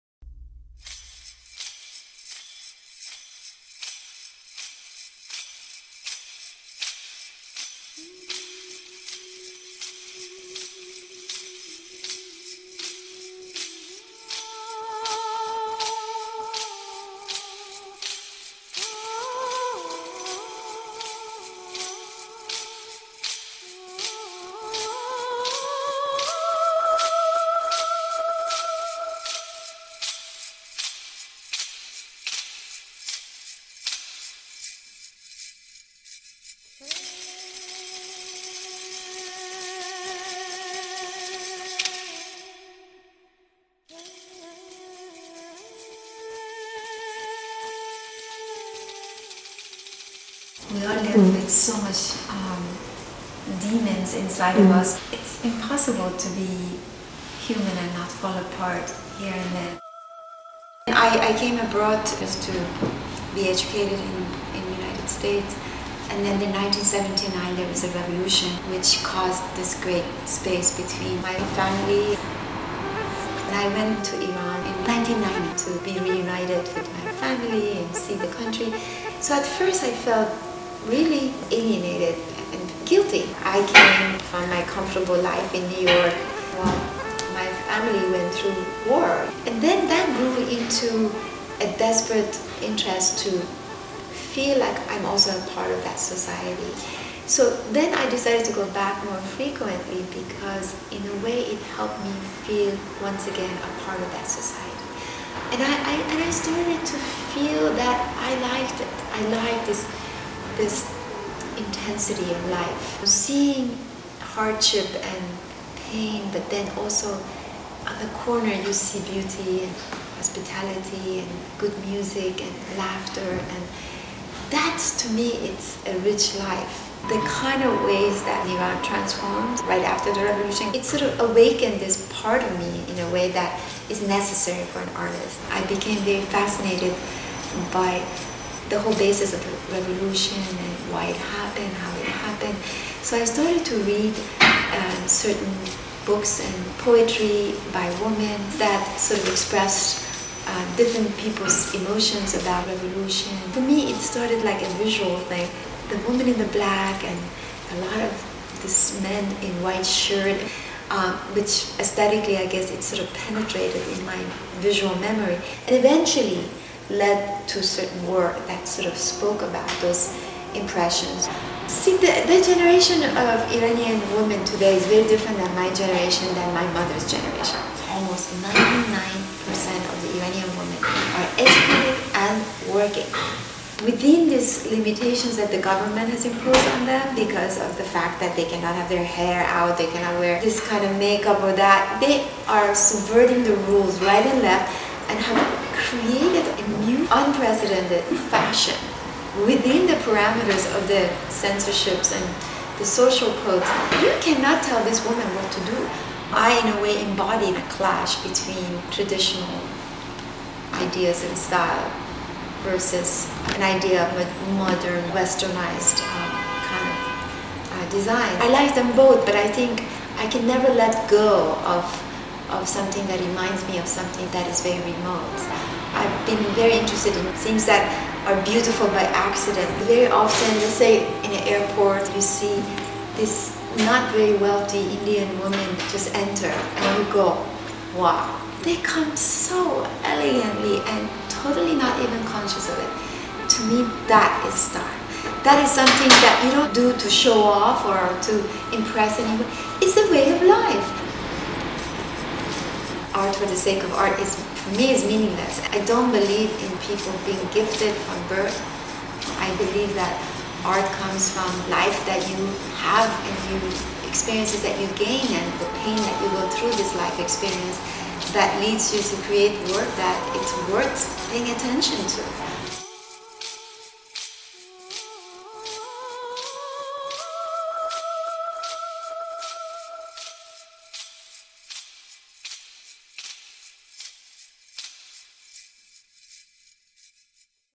Closets: Shirin Neshat for StyleLikeU.com
Shirin Neshat is an artist currently living in NYC.

 "Style is not about showing off," Shirin says, alluding to the elegant Indian women in saris that you see traveling in airports. It is the things in life that are beautiful by accident that she cherishes -- nothing mass-produced and nothing intended for an end result other than its original purpose. Identity before comfort, real before pretentious. "We all have demons living inside of us, it is impossible for us to be human and not fall apart, sometimes," Shirin so honestly confesses.

Shirin's deeply-rooted point of view is in everything from her signature indigenous jewelry and Cleopatra-inpsired eye makeup to her spellbinding artwork that divulges her passion for her Iranian origins and its cultural complexities. Exile from her homeland during the revolution, which included a Berkley college education, in addition to being brought up in pre-revolutionary Iran by parents with a modern outlook, has only magnetized her further towards an exploration of the authentic -- to wit, the historic splendor of the the Iranian people, its religious traditions, and the intensity of aliveness in revolutionary countries.

"I have always found women behind the veil striking," Shrin says. "I enjoy traditional clothes," as evidenced by her hand-embroidered jacket and the penchant for black as the background to her ancient jewels."These women's beauty wasn't bought," she declares.  "Their beauty was a part of their soul." And the soul is what Shirin has dedicated her life to investigating in her piercing, mesmerizing, and hypnotic photographic portraits that she then painstakingly emblazons with ancient Persian calligraphy.

"I love being among people who are living history," says Shirin. "My art focuses on revolution, women, and poetry. The veil is a symbol of women's repression, yet these women were effective in revolution, and poetry, too, which has come to the rescue in the face of brutality."  That's the reality of the Iranian society that Shirin gives oxygen to in her art, and is ultimately the kind of richness that upends entire nations. Shirin embodies the type of muse that we seek to put forth: Someone whose sense of self derives from things larger than herself. Selfies no where to be found.